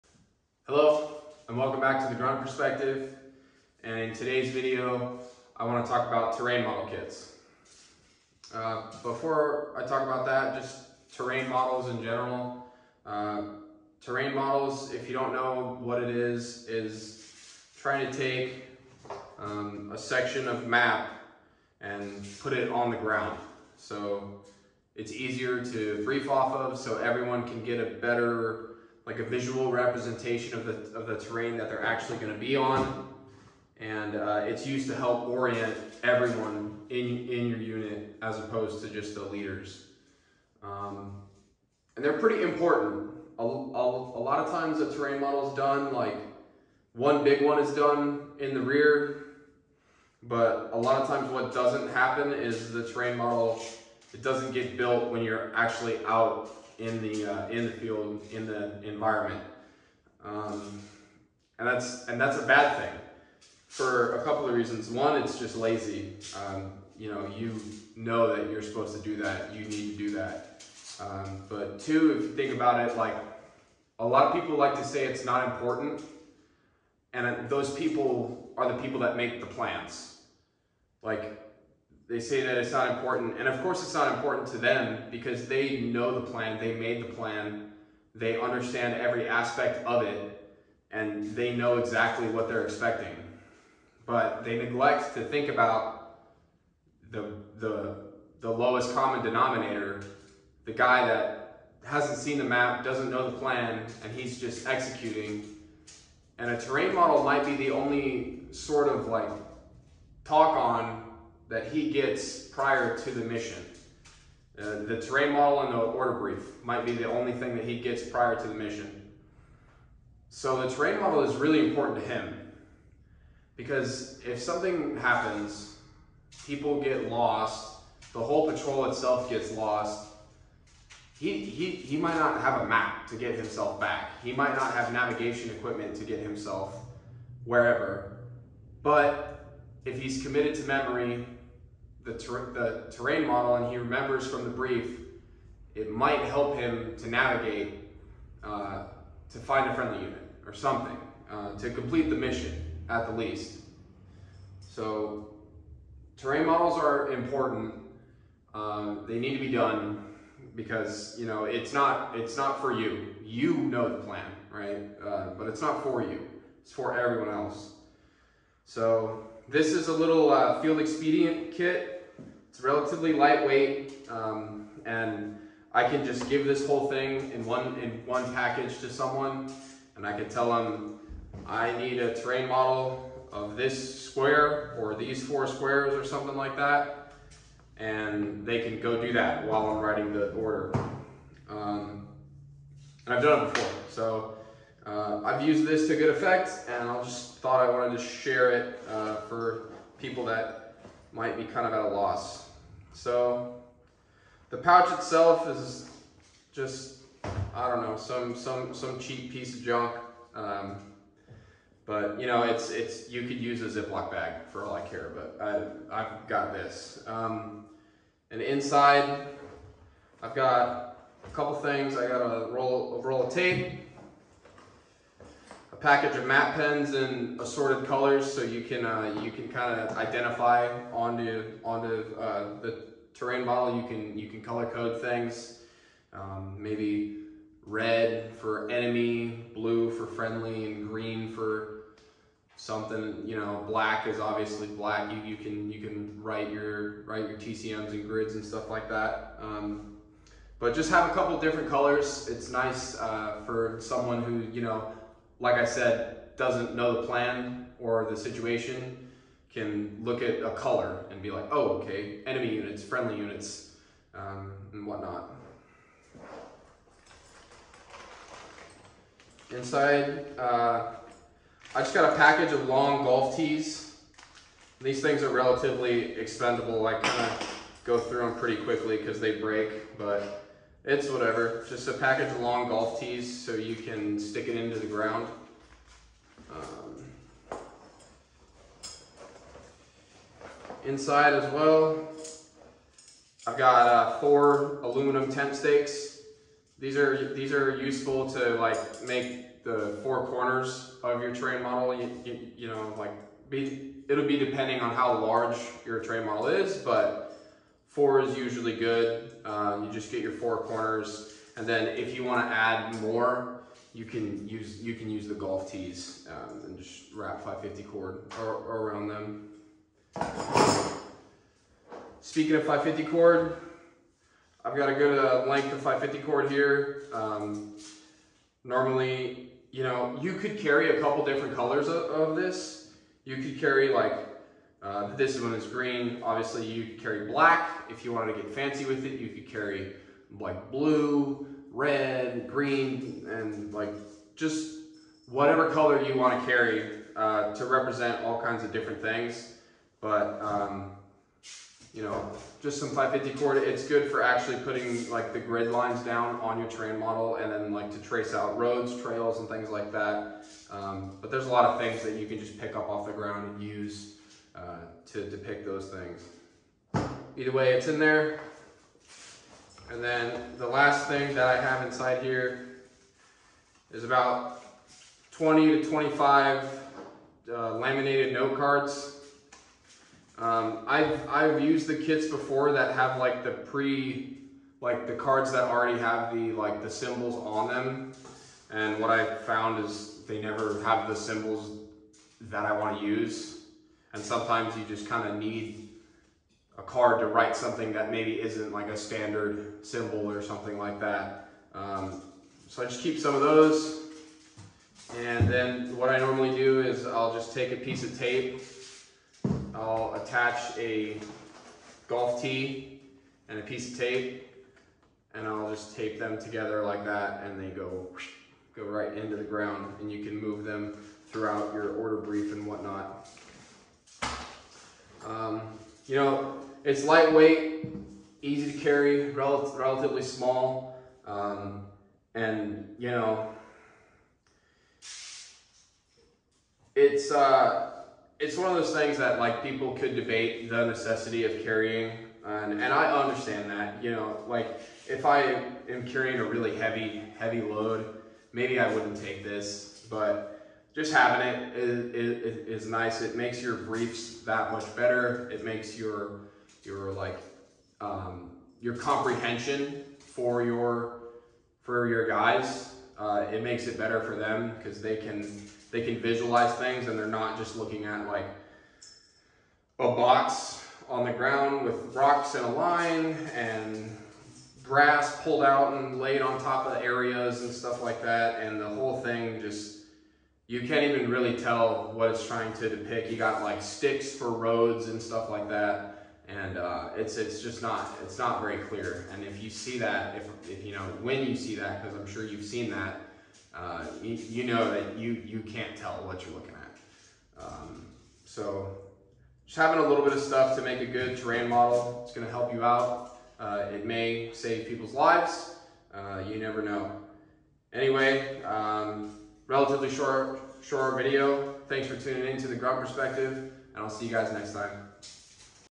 Terrain Model Kit
In this video I break down my terrain model kit and some reasons why terrain models are important.